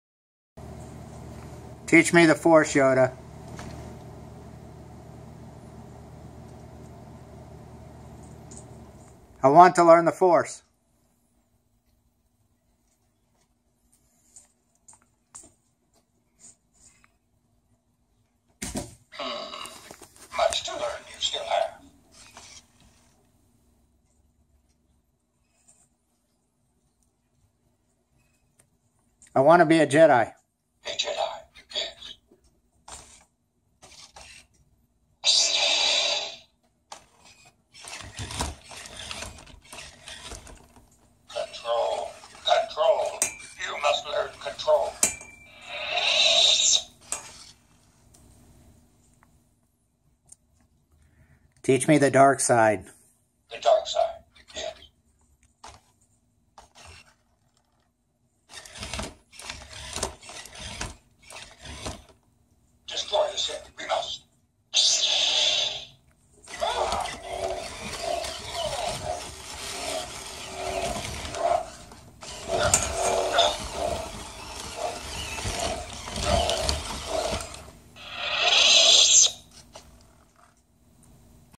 Cats versus Yoda 3
Yoda teaches kitty cats the force